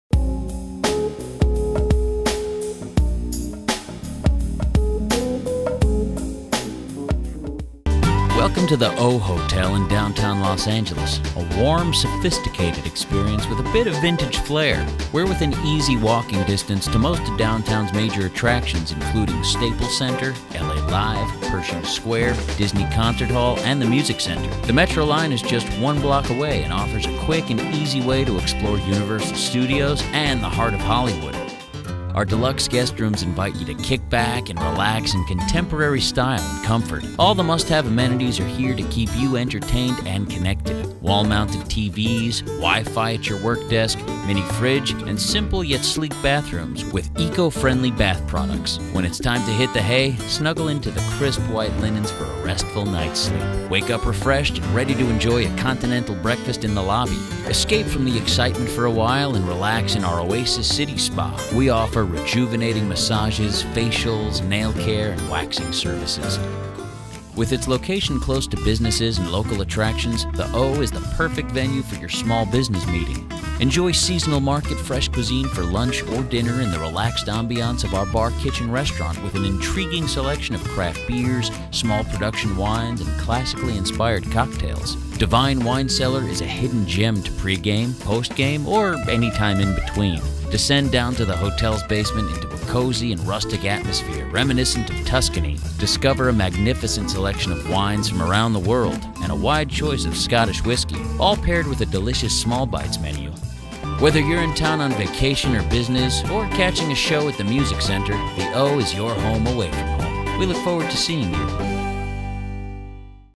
O Hotel
A serene haven for the business traveler, urbanite and out-of-towner, Orchid Hotel (O Hotel) offers 68 deluxe rooms with upgraded comforts, and gracious and efficient service expected of well-tended boutique establishments.